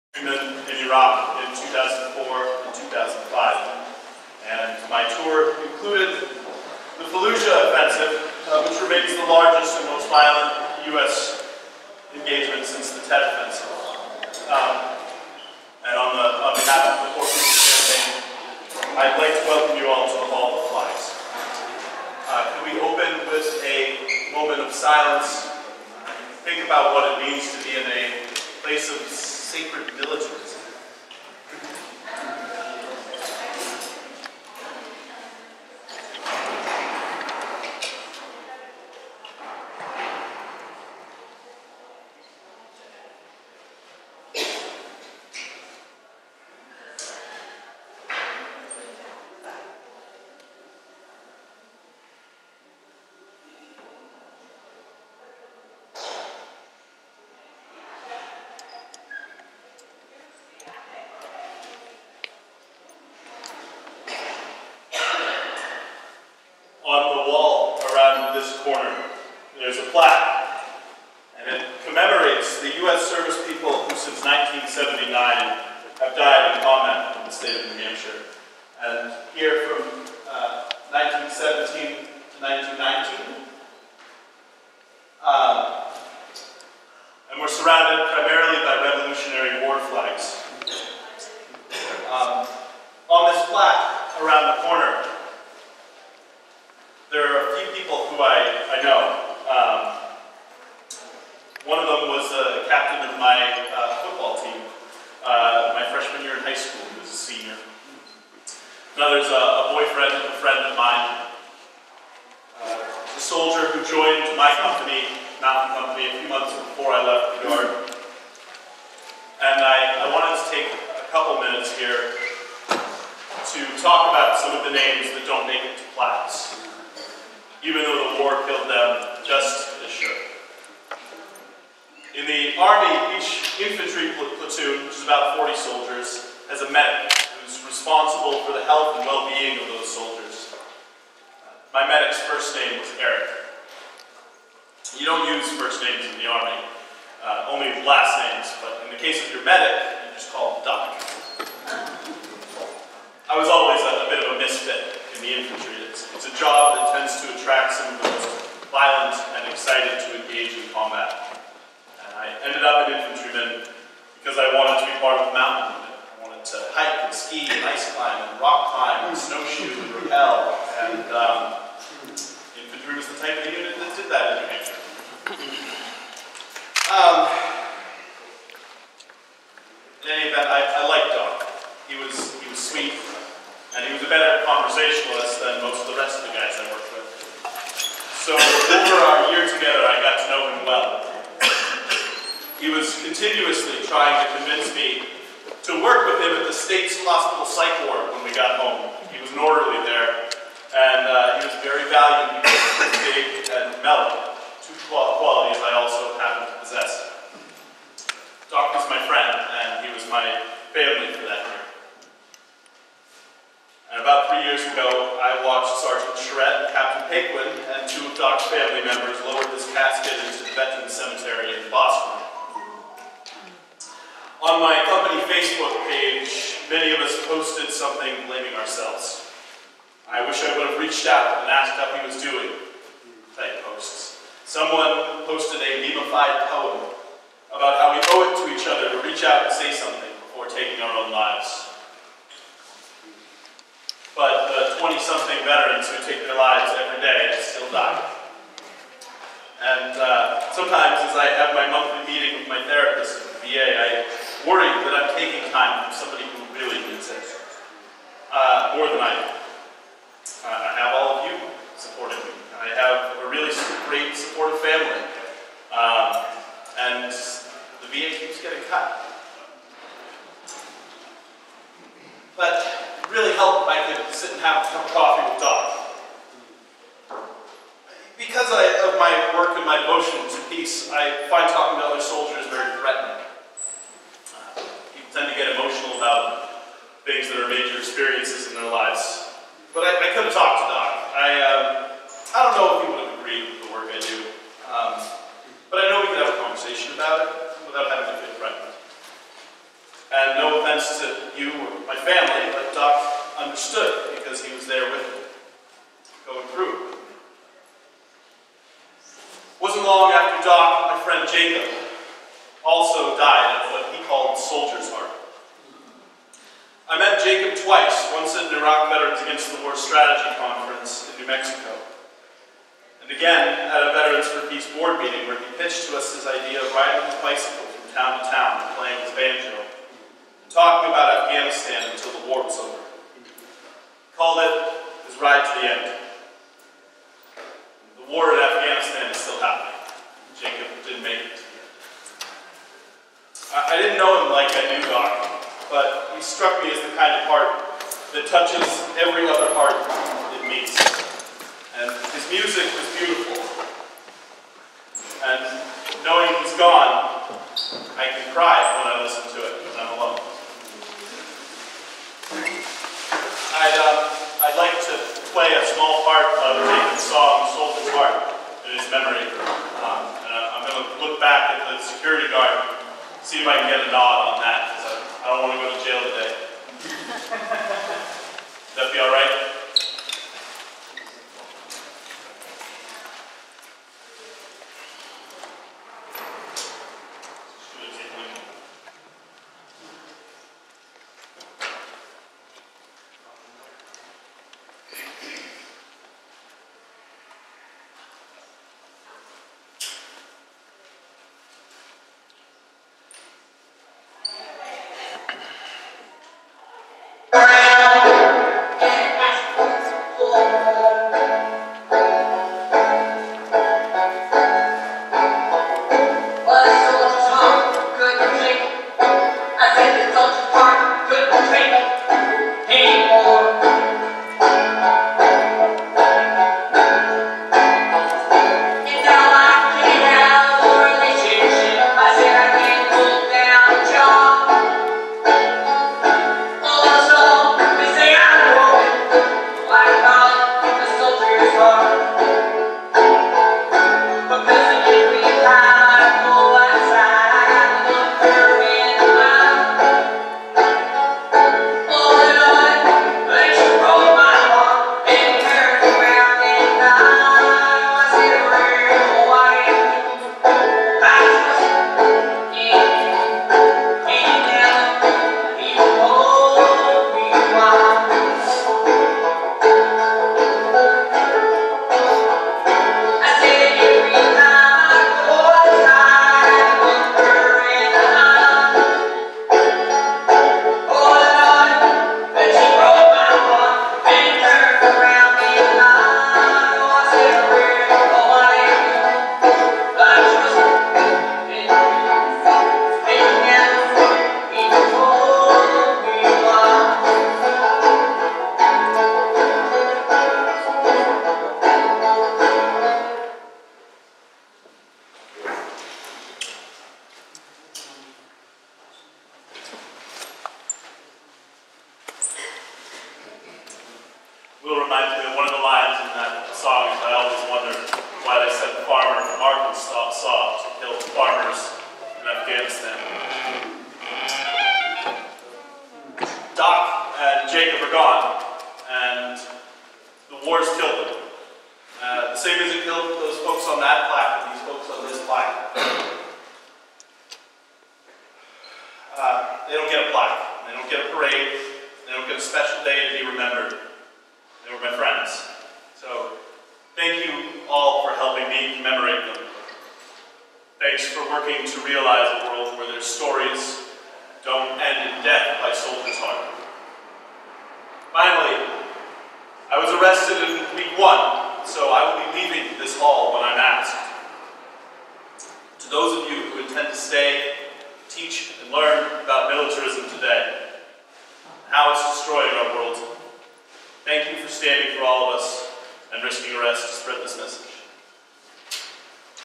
NH Poor People's Campaign Week 3 - Teach-in featuring Will Hopkins
On May 29, 2018, as a part of the Poor People's Campaign, activists occupied the Hall of Flags in the NH Statehouse to hold a teach-in protest about US militarism.

Here is Will Hopkins, executive director of NH Peace Action and member of Veterans For Peace, paying respect to two fellow soldiers and talking about how only some of the soldiers that gave their lives are memorialized on those walls.